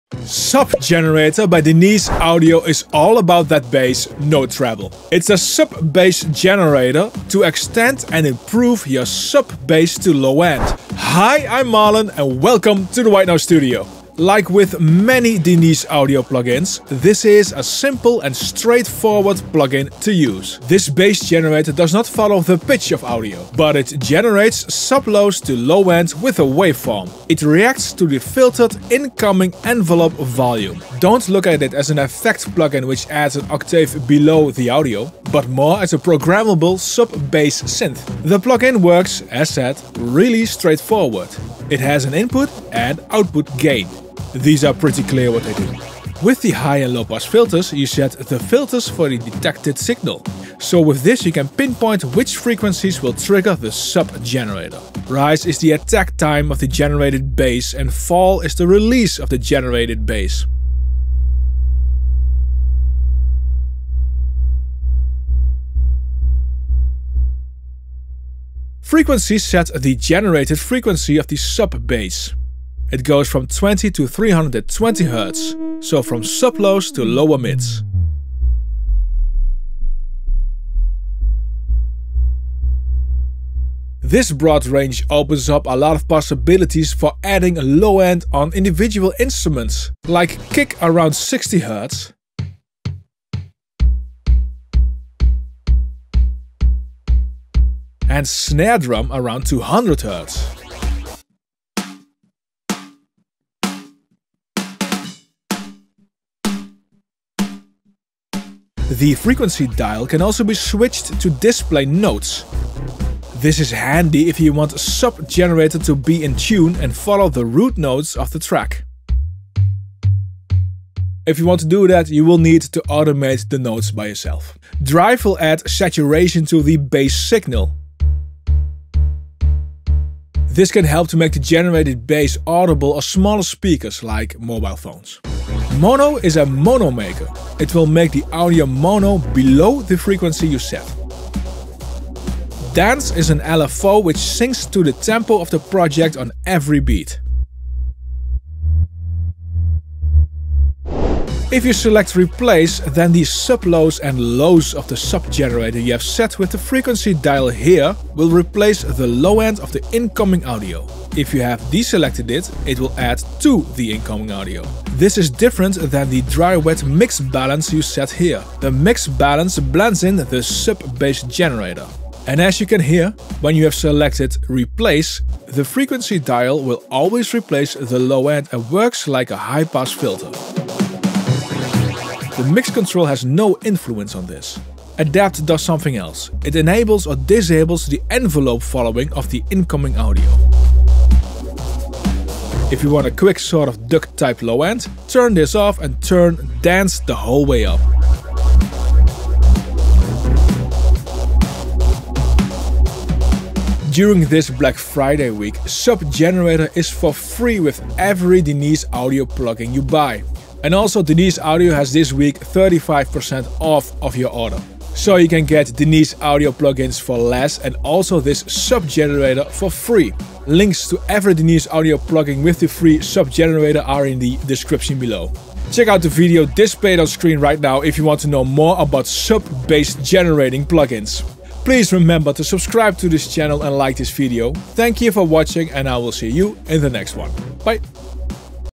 Expand your bass easily with Denise Audio Sub Generator - review
The Denise Audio Sub Generator is a sub bass enhancer / generator to beef up your low end.
It works by generating a low end waveform which can easily be blended in with your audio.

00:00 Intro
00:20 How Sub Generator Works
00:40 The Controls
01:28 Low end on individual intrument - kick + snare
01:49 Controls Continued.

Thanks!
Marlon